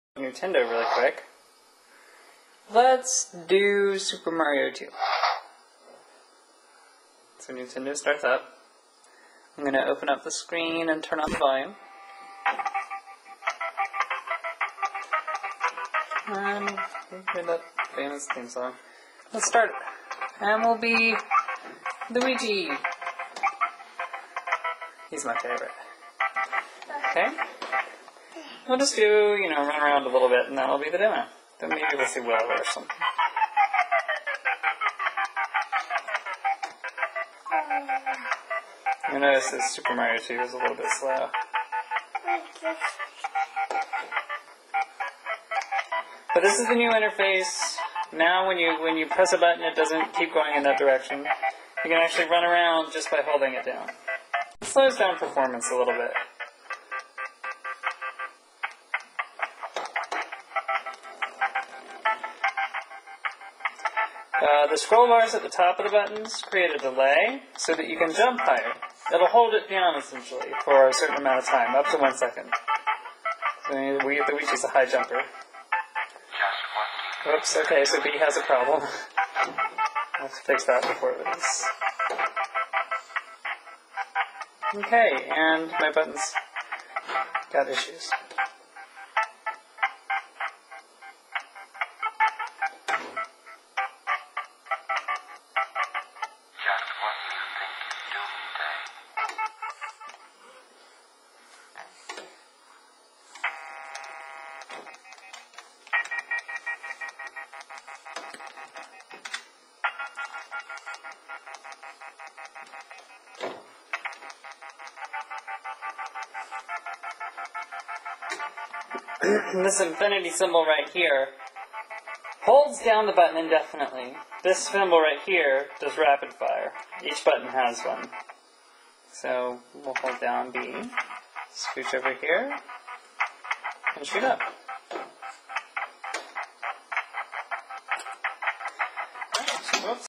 Newtendo Demo - Eagan Rackley
Demo video from a Newton Messagepad 2x00 emulator I wrote in the early 2000s. It was a great way to learn assembly on the 6502 and a little bit about early console architecture.

I've lost my password to the web page that hosted the project, but you can still visit it here: